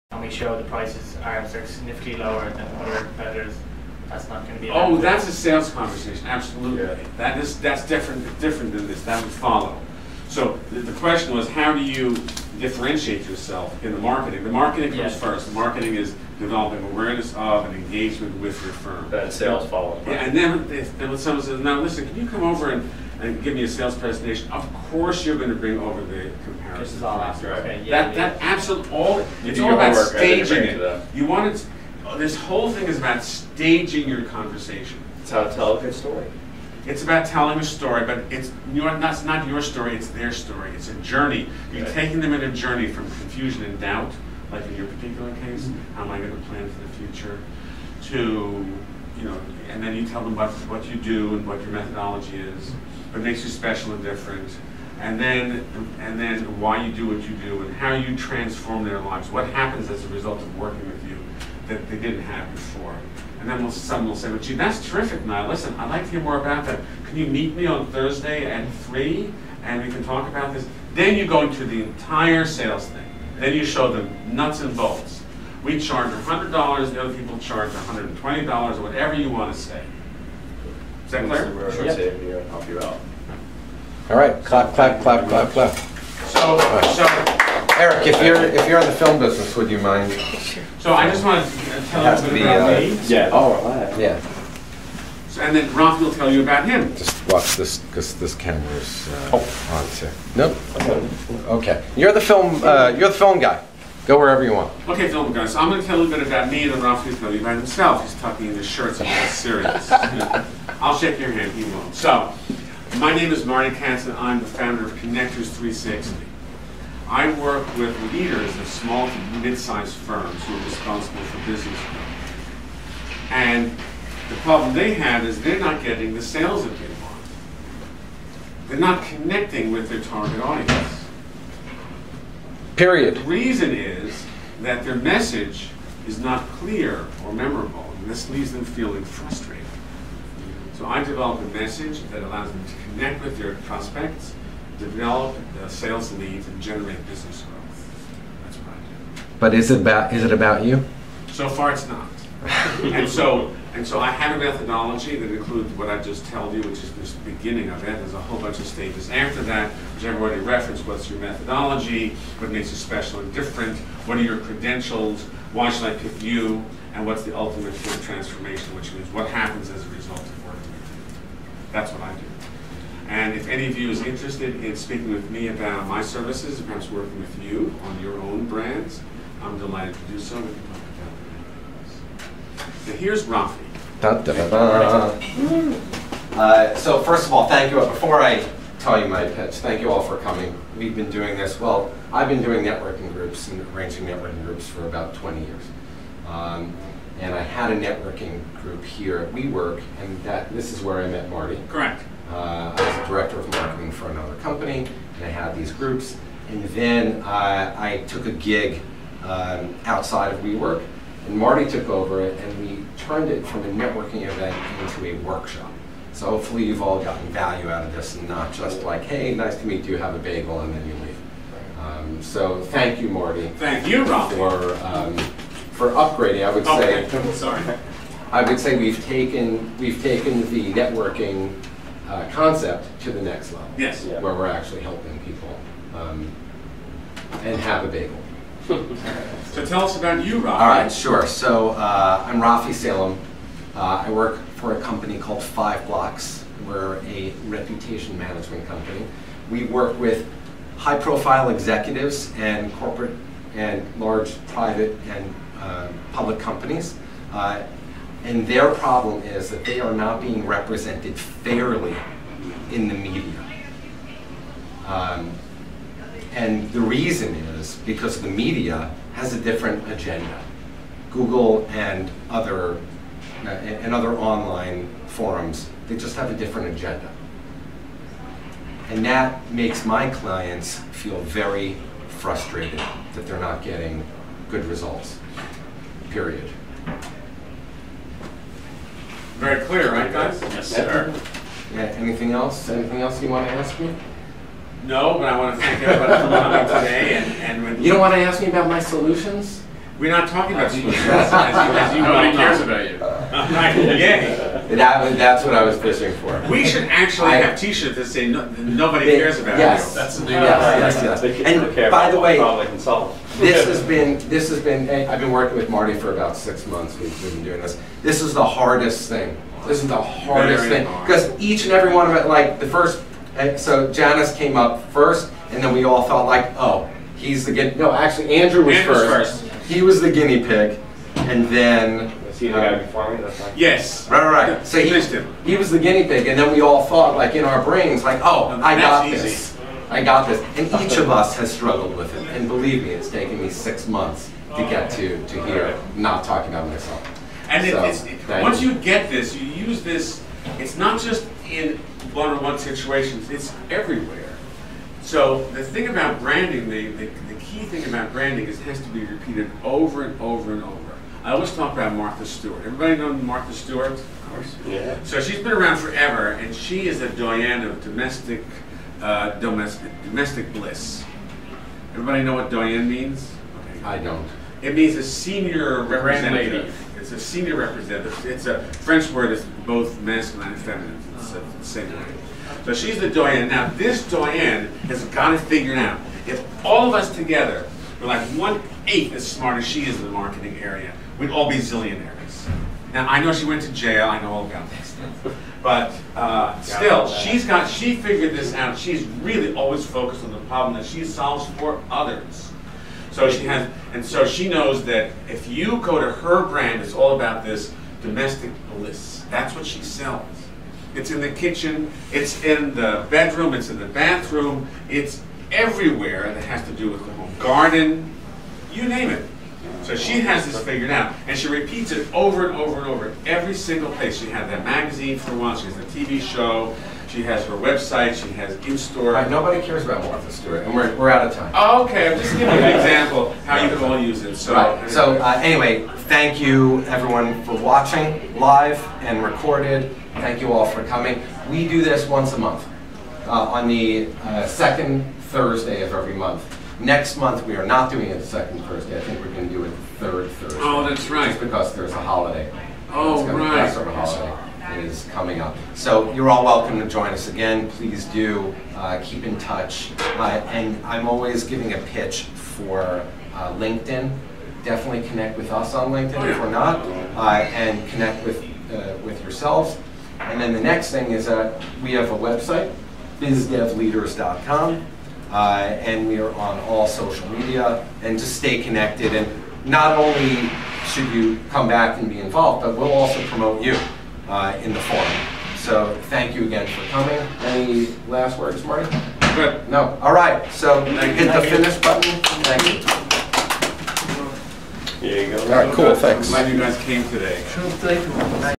Raphi Salem, Marty Katz, BizDev Leaders Messaging & Branding workshop at WeWork 03 12 2020 part 3/3
Raphi Salem, Marty Katz, BizDev Leaders Messaging and Branding workshop at WeWork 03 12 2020 part 3/3
Learn the 3 rules of marketing and apply to your own business.